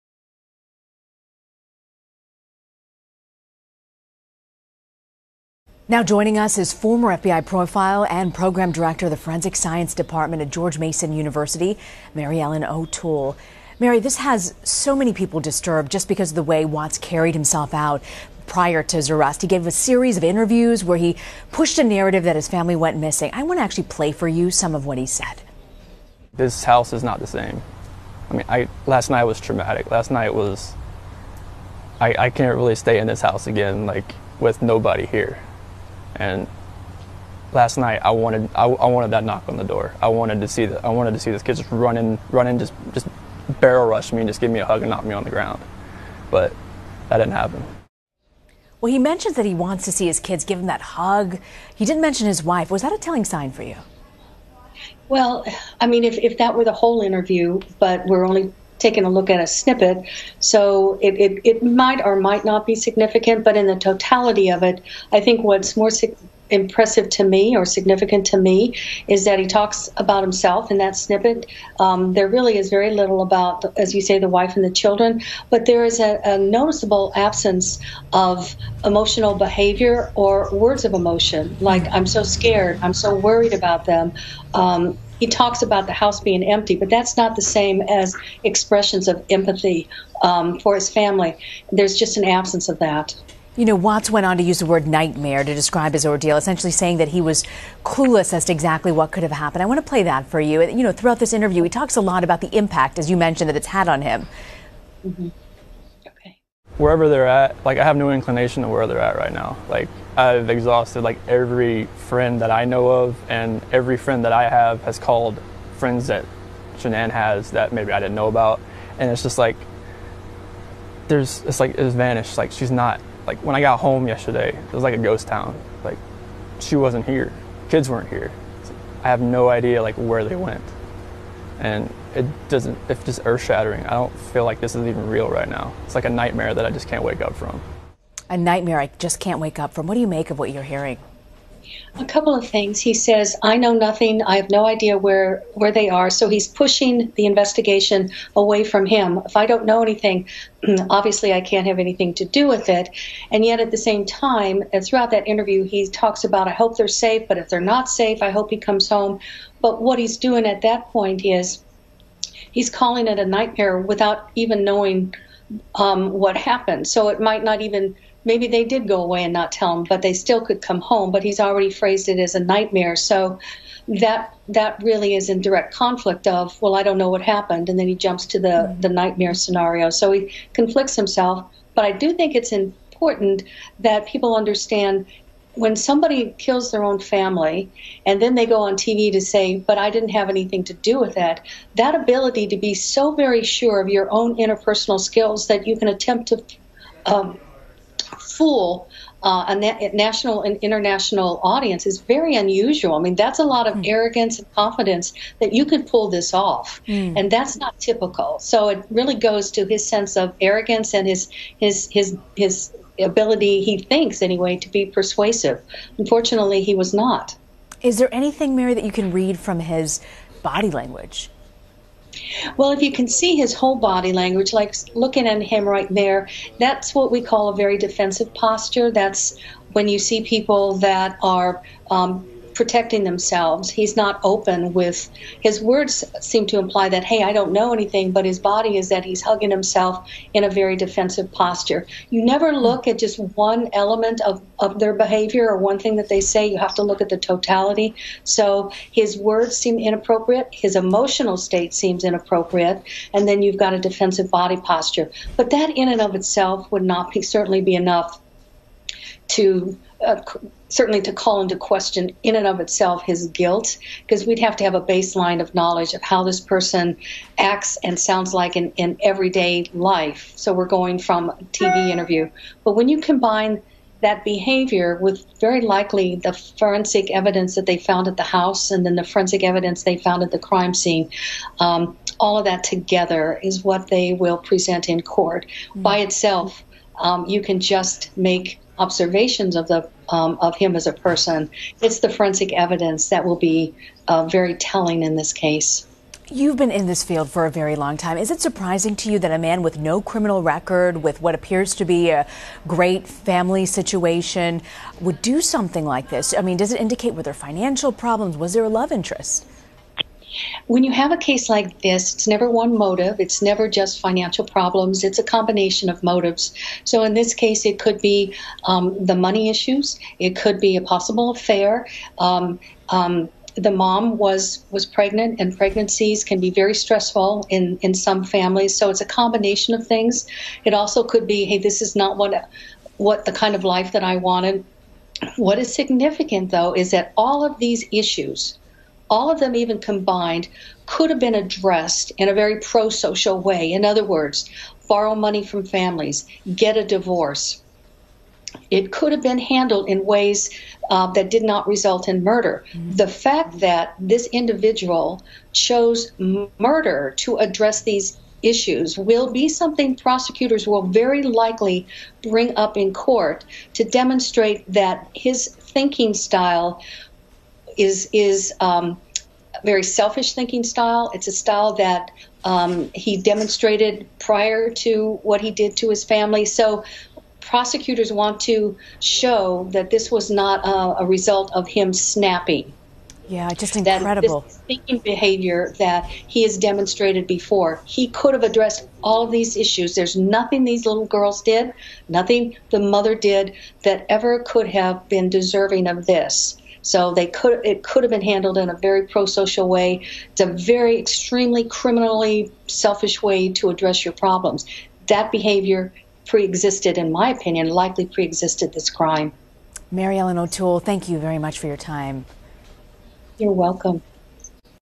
FBI Profiler Examines Chris Watts Porch Interview
Chris Watts murder of his family has prompted a ton of speculation. What did the professionals see?